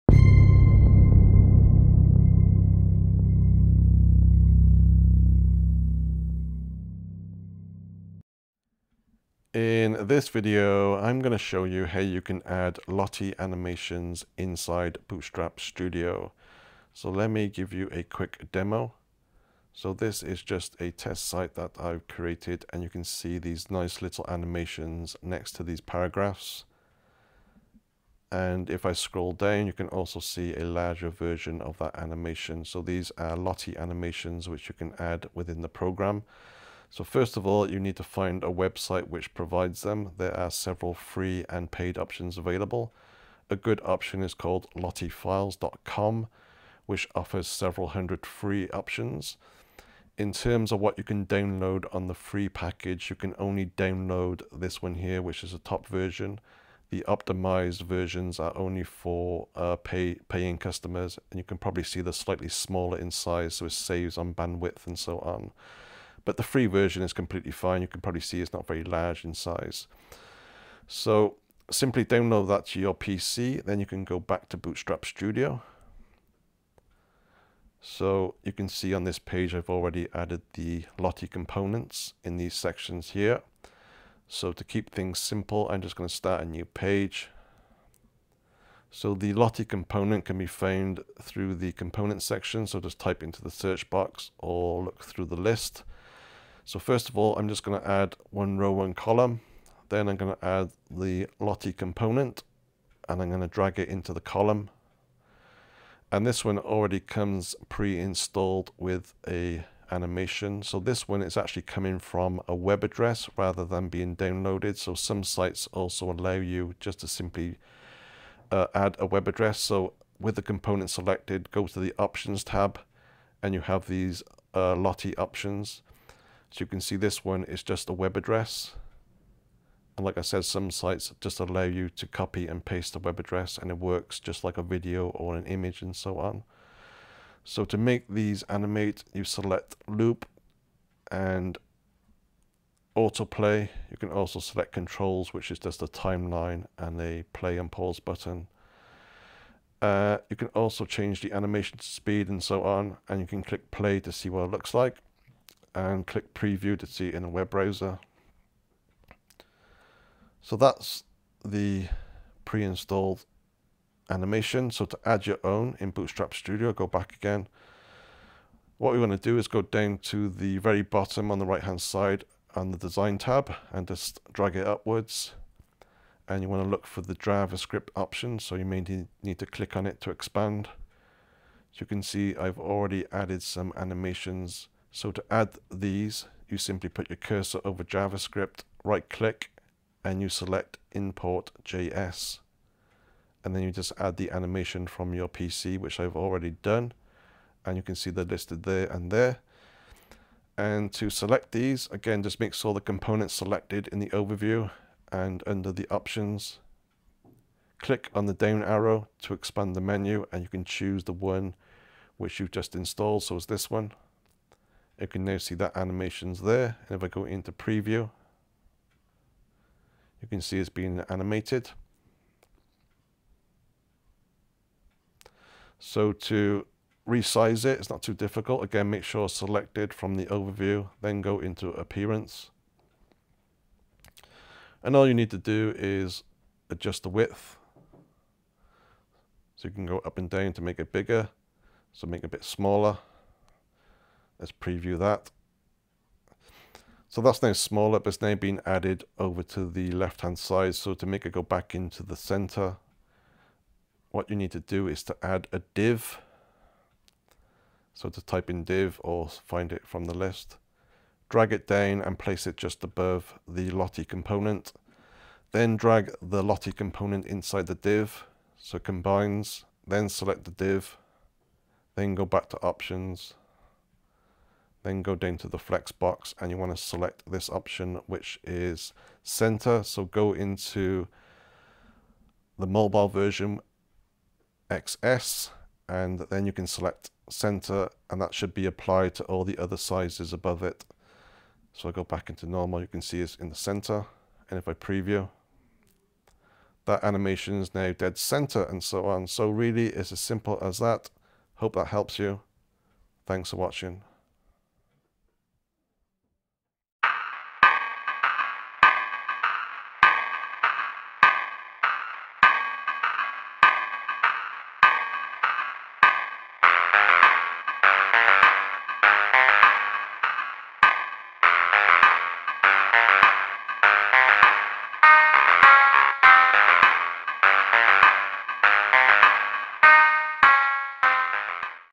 How to Add Lottie Animations inside Bootstrap Studio
Lottie Animations, as the name suggest, are images which animate inside websites. You can easily add these inside bootstrap studio, by selecting the "Lottie" component from the component list.

There are several sites which provide these animations, both free and paid options. In some cases, some sites allow you to simply add a URL within bootstrap studio, which then displays the animation without installing anything. You can also download the animation, and add it in the JavaScript section, by right clicking and select Import JS. You can then choose your animation from the options section.